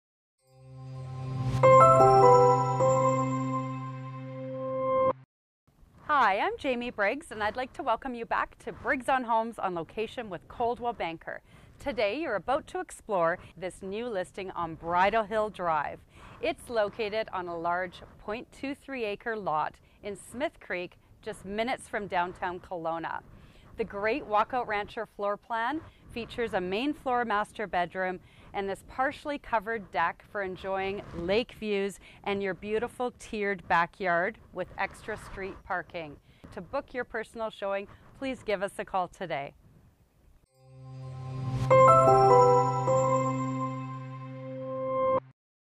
NOW SOLD! 3057 Bridlehill Drive, Smith Creek/West Kelowna $689,000
Located on a large .23 acre tiered lot in Smith Creek this beautiful walkout rancher features a partially covered deck with scenic lake & valley views, backyard access for extra parking and a bright walkout basement.
The open concept main floor greets you with bright open space, semi vaulted ceilings and kitchen with ample shaker style cabinets and raised eating bar.
You'll enjoy the scenic views from the dining room and living room with gas fireplace. The partially covered back deck has gas hook-up for BBQ and also access to the large main floor master bedroom with 5-piece ensuite. Also on the main is a 2nd bedroom, full bathroom, laundry, and den/office.
Downstairs the bright walkout basement features 2 additional bedrooms (one connected to a full bathroom), huge fully finished storage room (or media room) plus covered lower patio complete with hot tub. You'll love this quiet neighborhood and large lot complete with backyard access from the street below for extra parking!
For more information on Kelowna Real Estate please call the Briggs On Homes Real Estate Group of Coldwell Banker Horizon Realty at , or go

3057 Bridlehill Drive, West Kelowna
$689,000
MLS 10158789